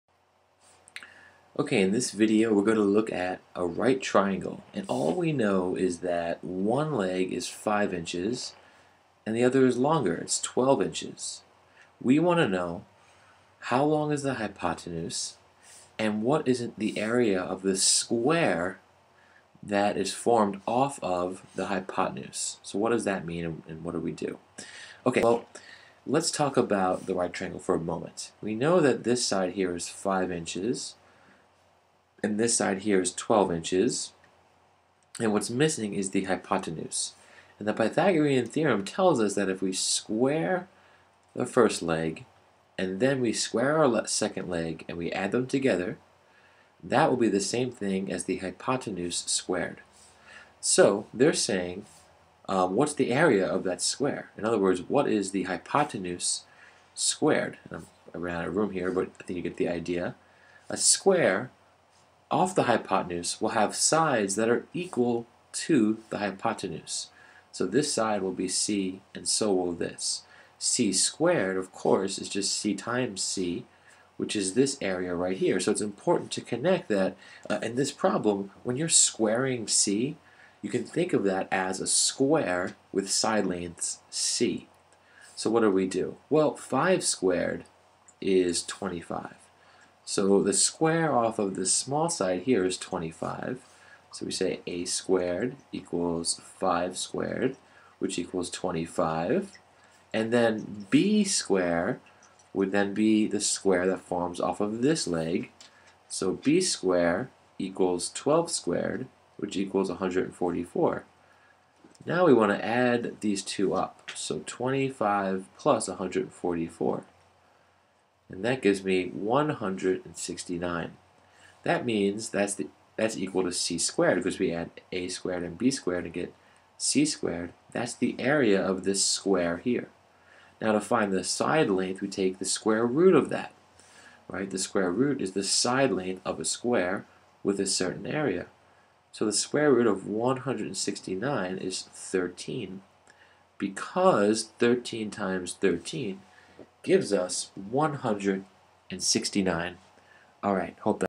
Hypotenuse Squares
A worked example of the pythagorean theorem